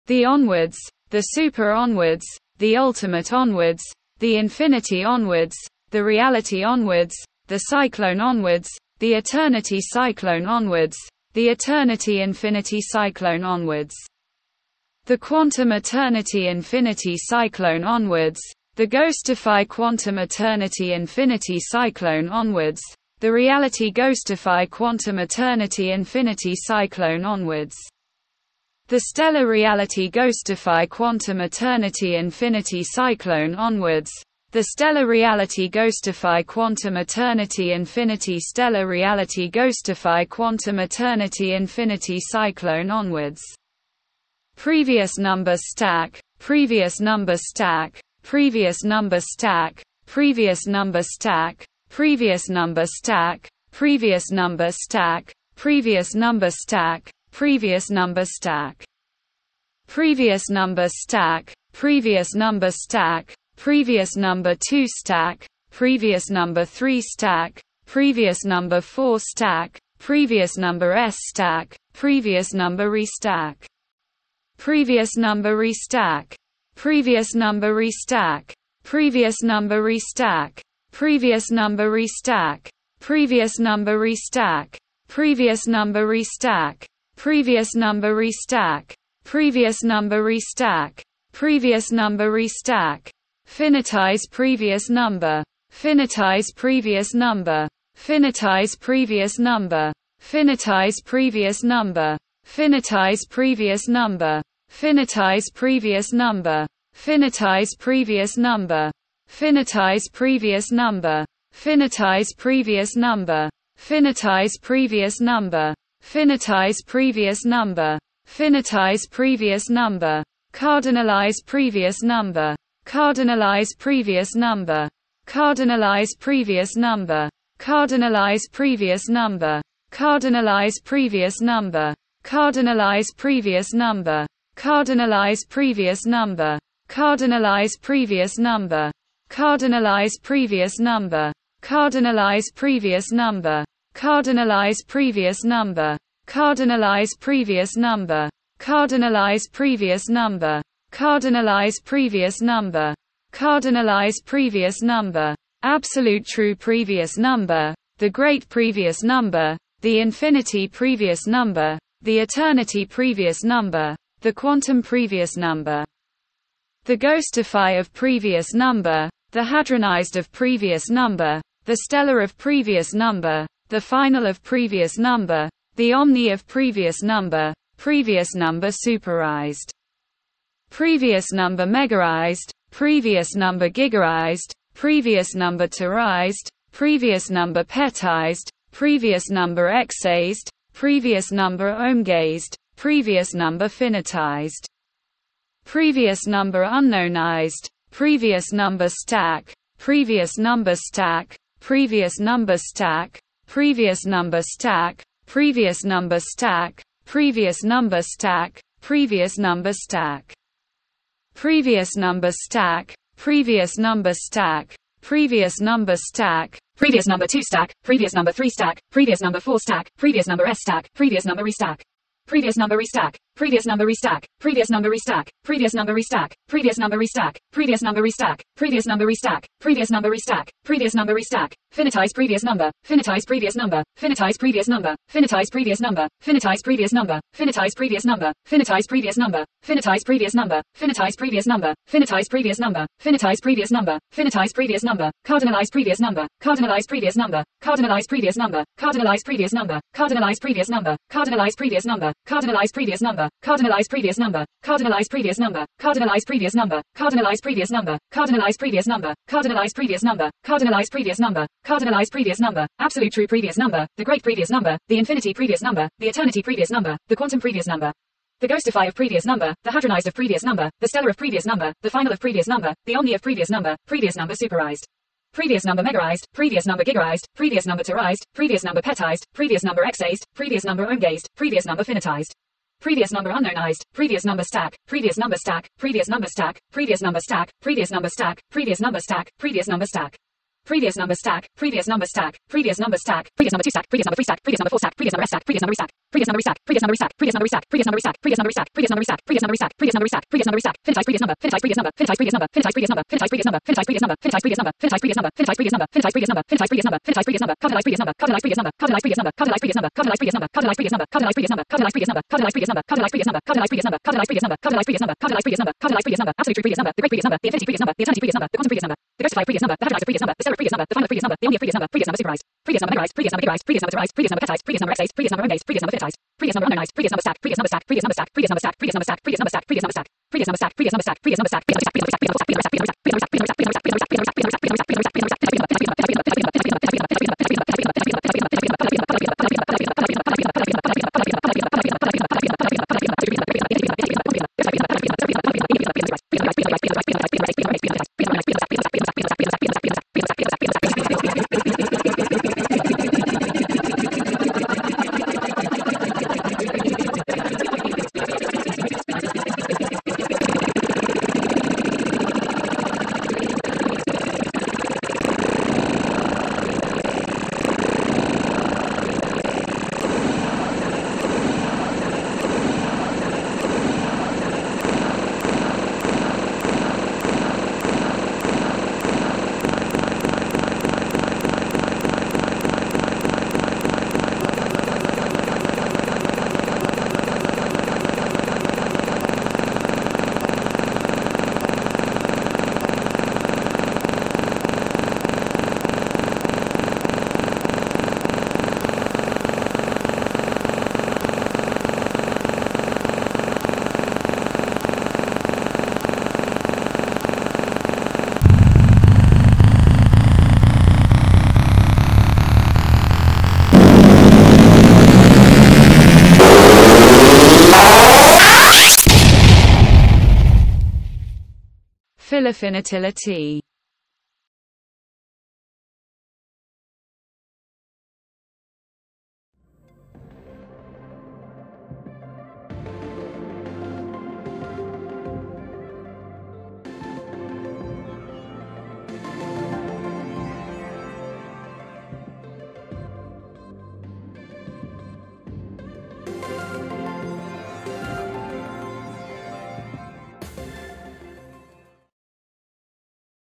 Cycle #7 with British spellings
In fictional googology, the study of made-up large numbers, several functions for making numbers bigger, especially those in cycles, consist of terminology relevant to the section of the video that the cycle represents with the "-ize" suffix attached. However, since the "-ize" suffix in American English becomes "-ise" in British English and other dialects, I wanted to make a proof of concept to show functions with the latter version of the suffix as a proof of concept, despite being American myself. I decided to use Cycle #7 from Numbers 0 to ??? [2] by @notnt2048 to do this, the timestamped link to which is
The background audio is the English text-to-speech voice from the .co.uk version of Google Translate reading off the entries of the cycle. This was inspired by the AI Speaking Numbers 0 to ? series by @Jesse_Kohn. The entries themselves were created in Microsoft PowerPoint, and this video is an edited screen-recording of me stepping through the slideshow while the TTS reads off the entries in the background.

This video's creation concluded on 3 May 2024